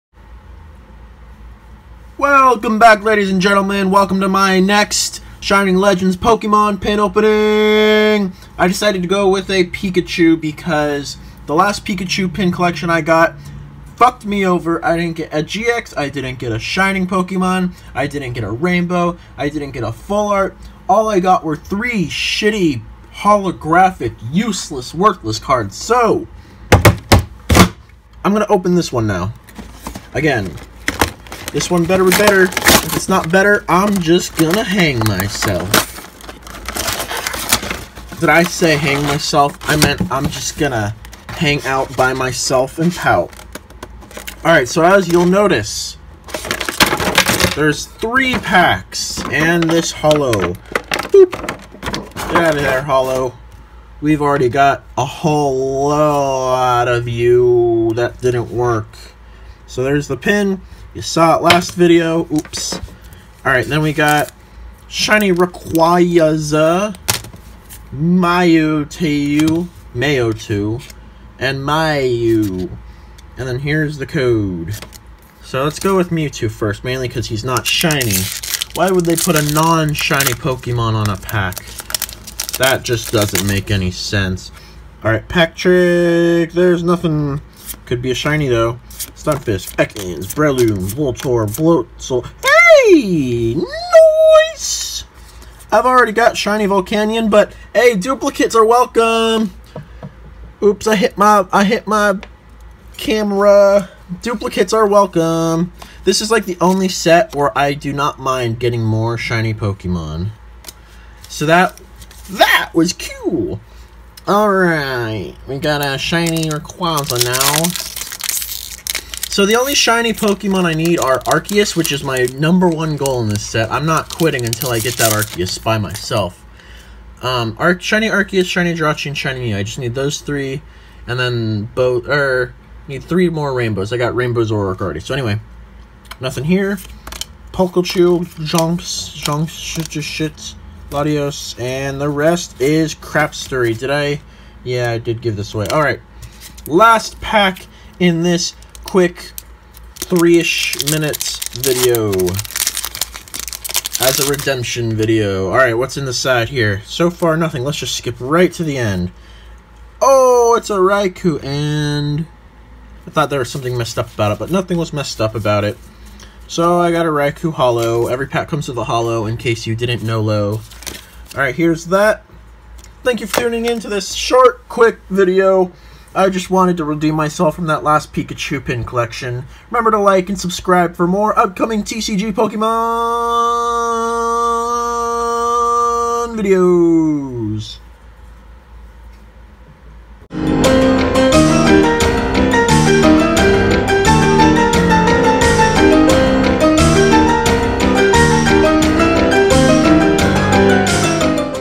LETS TRY THIS AGAIN... Pokemon Pikachu Shining Legends Pin Collection Opening #2
Trying again after my first Pikachu Pin Collection Box was a dud.